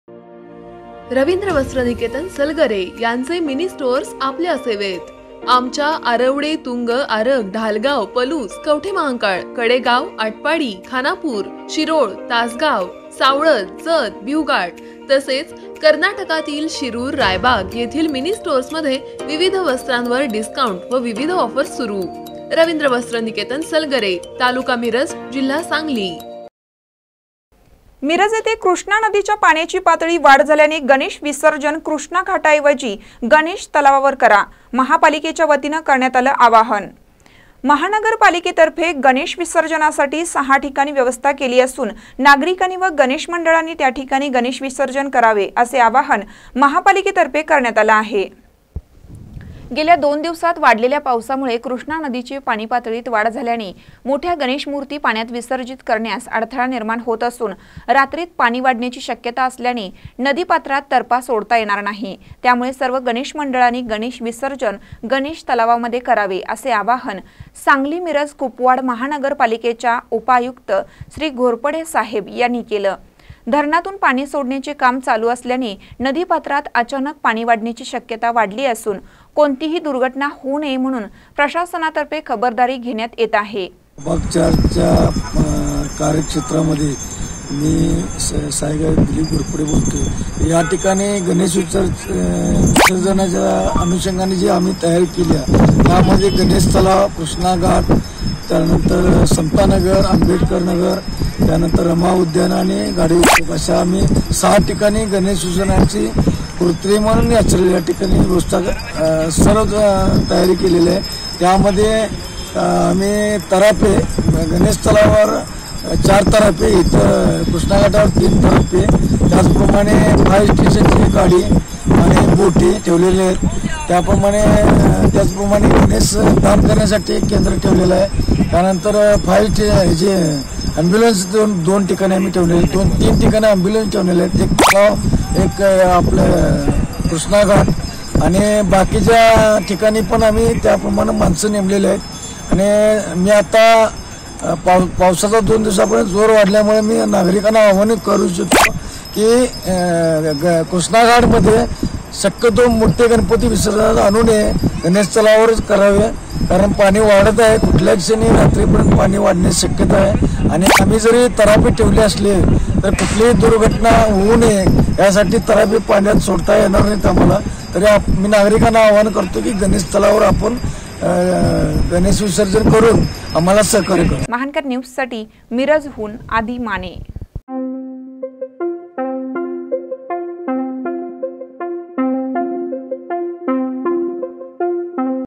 मिरज येथे कृष्णा नदीच्या पाण्याची पातळी वाढ झाल्याने, गणेश विसर्जन कृष्णा घाटा ऐवजी गणेश तलावावर करा
मिरज येथे कृष्णा नदीच्या पाण्याची पातळी वाढ झाल्याने, गणेश विसर्जन कृष्णा घाटा ऐवजी गणेश तलावावर करा, महापालिकेच्या वतीने करण्यात आल आवाहन